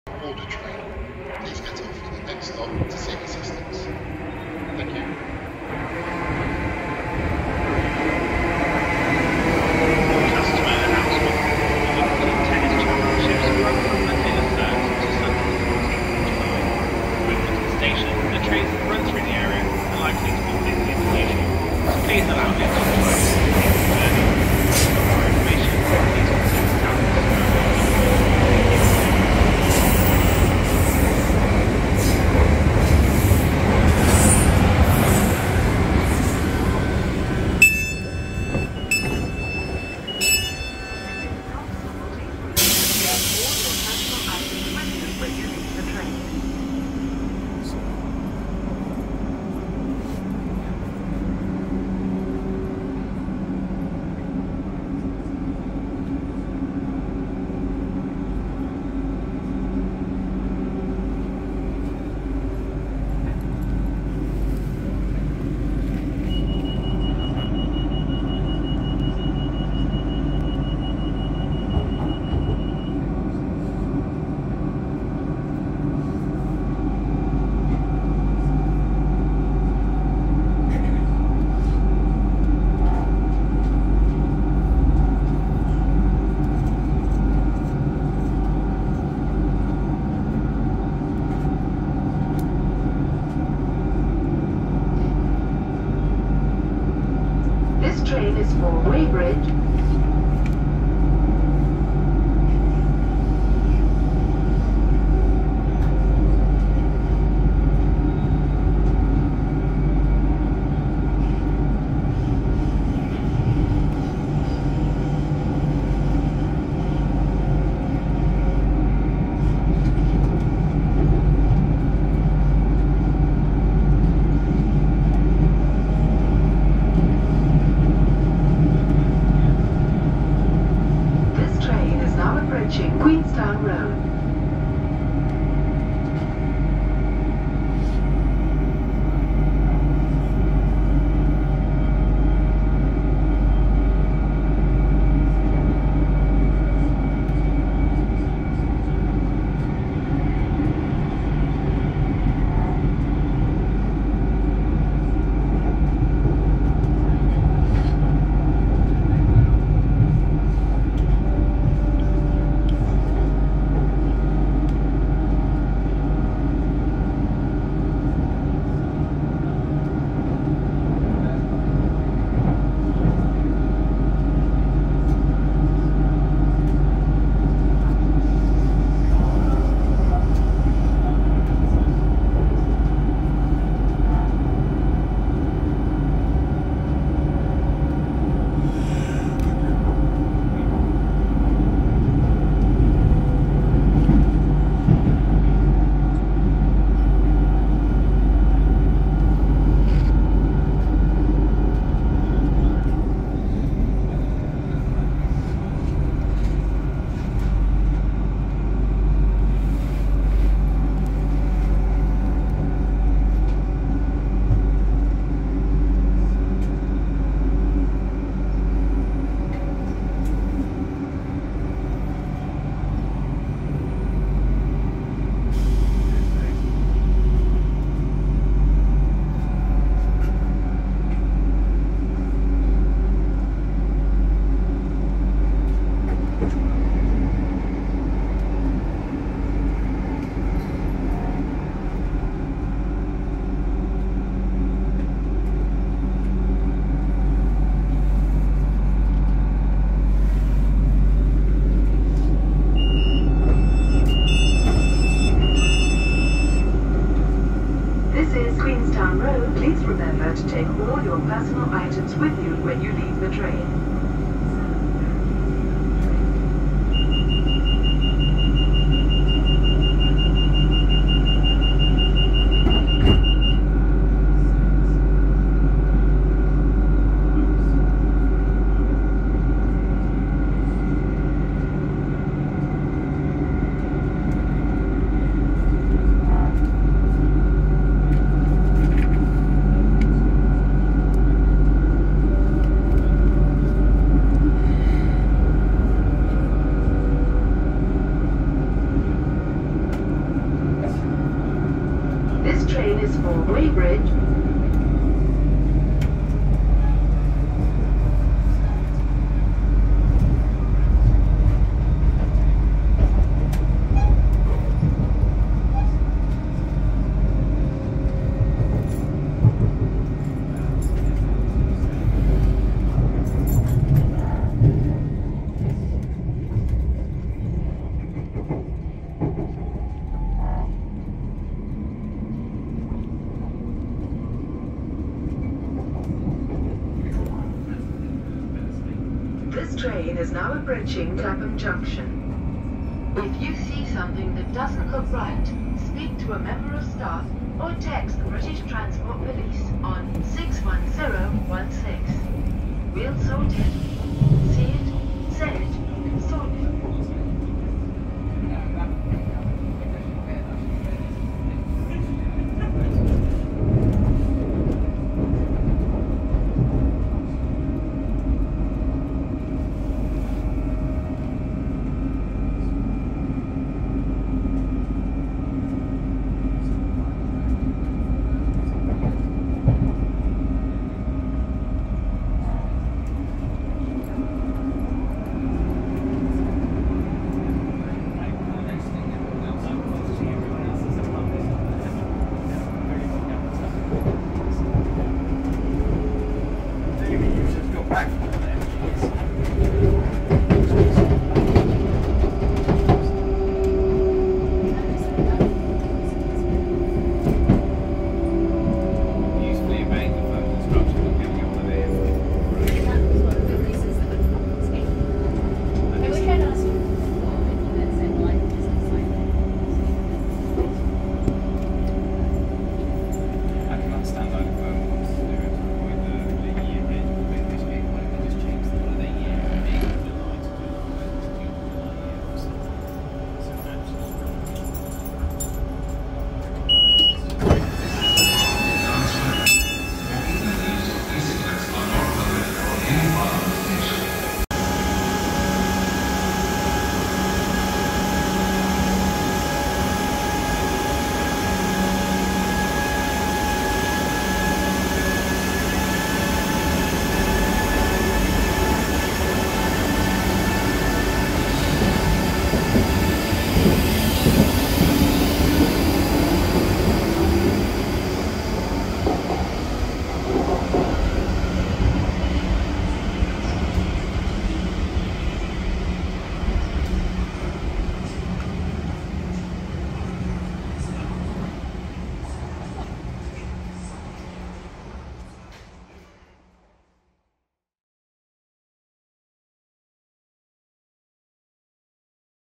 South Western Railway Class 458/4 ride in service - Vauxhall to Clapham Junction
On the 25th June 2024, South Western Railway entered its refurbished Class 458/4s into passenger service on London Waterloo-Weybridge services via Staines. Join me in this video where we take a look at SWR's refurbished units onboard Class 458415 and 458421 on their very first passenger service post-refurbishment, on a short run from Vauxhall to Clapham Junction.

Corrections
00:54 The unit we are riding on is 458421, not 458422. Apologies for any confusion.